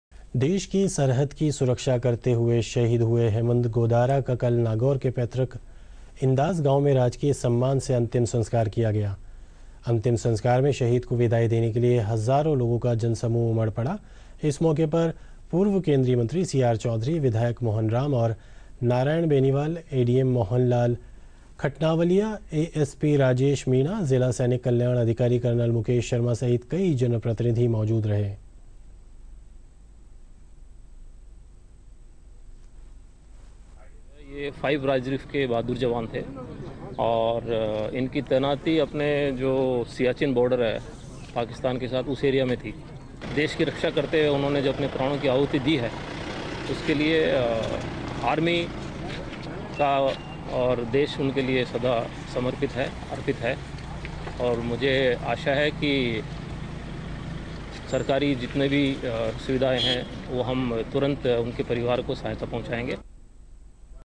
हेमेन्द्र गोदारा का कल पैतृक इंदास गांव में राजकीय सम्मान से अंतिम संस्कार किया गया : नागौर
देश की सरहद पर सुरक्षा करते हुए शहीद हुए हेमेन्द्र गोदारा का कल नागौर के पैतृक इंदास गांव में राजकीय सम्मान से अंतिम संस्कार किया गया। अंतिम संस्कार में शहीद को विदाई देने के लिए हजारों लोगों का जनसमूह एकत्रित था। इस मौके पर, पूर्व केन्द्रीय मंत्री सी.आर.चौधरी, विधायक मोहन राम और नारायण बेनीवाल, एडीएम मोहन लाल खटनावलिया एएसपी राजेश मीणा, जिला सैनिक कल्याण अधिकारी कर्नल मुकेश शर्मा सहित कई जनप्रतिनिधि मौजूद रहे।